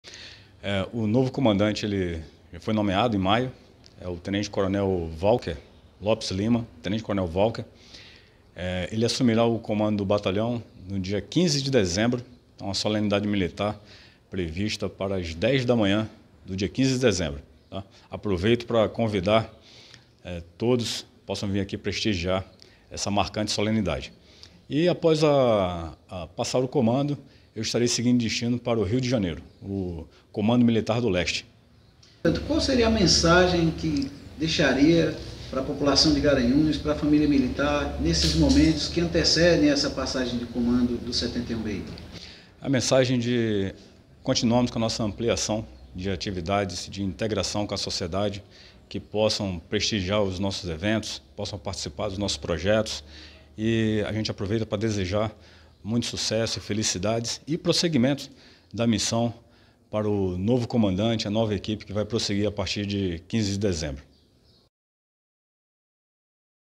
PRÓXIMO DE CONCLUIR COMANDO, CORONEL BRITO É AGRACIADO COM CIDADANIA GARANHUENSE
Próximo de concluir a missão de comandar o 71º BI Mtz, Batalhão do Exército sediado em Garanhuns, o Coronel Brito foi agraciado com o Título de Cidadania Honorária de Garanhuns. A oficialização da honraria aconteceu na manhã desta terça-feira, dia 12, pela vereadora Nelma Carvalho (PTB), autora da proposição na Câmara Municipal.

De acordo com Nelma, a honraria foi aprovada por unanimidade de votos pelos demais Parlamentares que integram a Casa Raimundo de Moraes.

“Não se trata apenas de mais um Comandante que passa pelo Batalhão do Exército em Garanhuns, e sim um Militar que promoveu uma verdadeira integração entre a Unidade e a Sociedade, sobretudo de forma solidária, garantindo ações e alimentos para famílias carentes, através das realizações sociais do 71º BI Mtz. Estou feliz por propor essa homenagem em nome de todos os garanhuenses”, ressaltou Nelma Carvalho, que na ocasião, também entregou ao Coronel Brito, um Voto de Aplausos conferido ao Batalhão pelo desenvolvimento do projeto Quartel de Portas Abertas.

O Coronel Ronny de Brito Barros está à frente do Batalhão Duarte Coelho desde janeiro de 2022. Ele é natural de São Lourenço da Mata e ingressou no exército em 1994, tendo sido promovido ao atual posto de Coronel em abril do ano passado. Antes do 71º BI Mtz atuou em Unidades Militares localizadas em Jaboatão dos Guararapes-PE; Tefé-AM; Marabá-PA; Aracaju-SE e no Rio de Janeiro. A data e local de entrega do Título será anunciada nos próximos dias.

PASSAGEM DE COMANDO – E o 71º BI Mtz já trabalha visando a passagem de comando da Unidade, que acontecerá no próximo dia 15 de dezembro, durante solenidade na própria Unidade Militar, com início previsto para às 10h.  O novo comandante da Força de Ação Rápida do Nordeste será o Tenente-coronel Walker Lopes Lima. Já o Coronel Brito passará a servir no Comando Militar do Leste, no Rio de Janeiro.

12/09/2023.